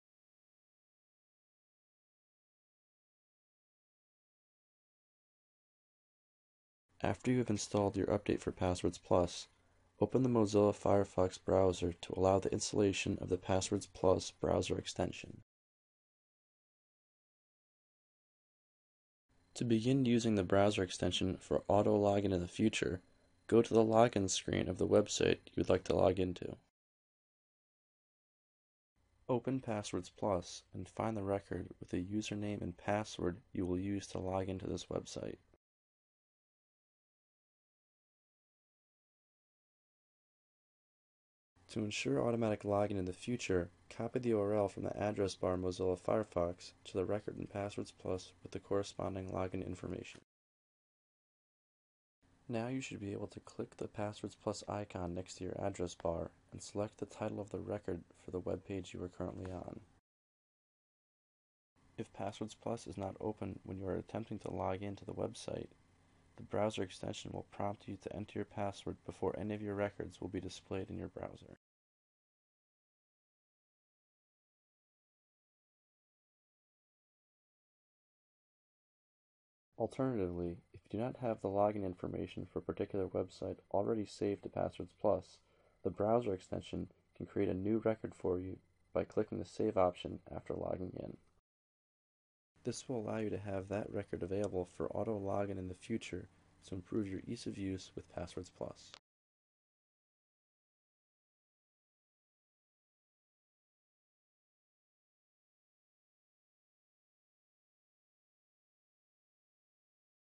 Passwords Plus for Windows - Firefox Browser Extension
See the new Passwords Plus browser extension for Firefox in action! Now securely auto-fill and save web logins with the Windows version of Passwords Plus. And... updates made in the Passwords Plus browser extension will automatically be reflected on all the devices where Passwords Plus 2.0 is owned.